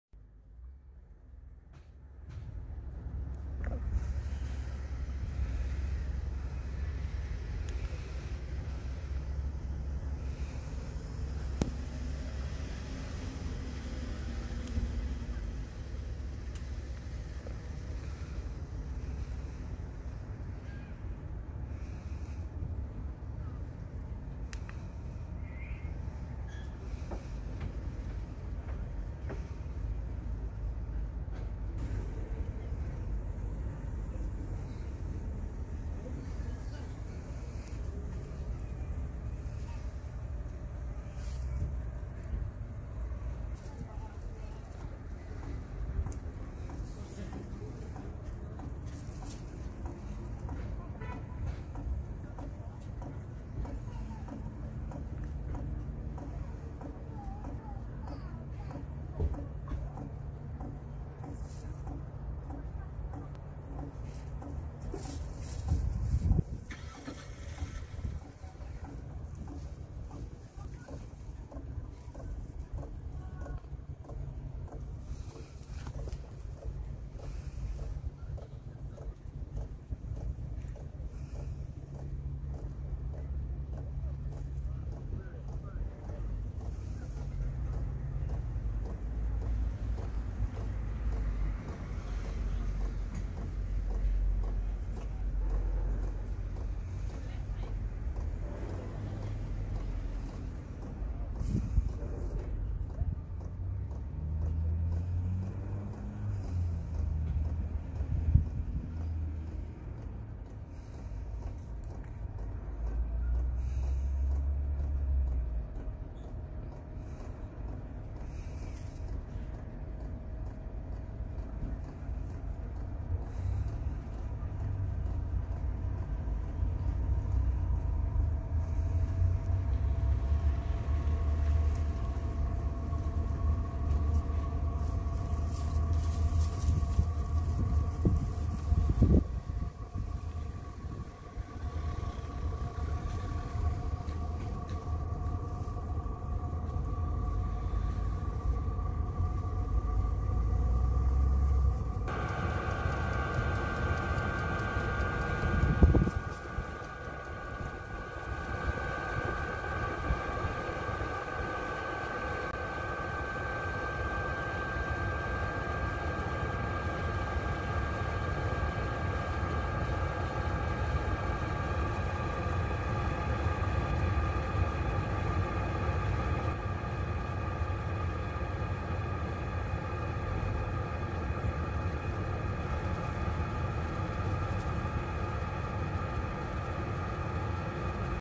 최신형 무시동히터 사용후기 😄
무시동히터 사용후기입니다 (5kw)
소음을 제일많이들 궁금해하시니
한번봐보세요~ ^^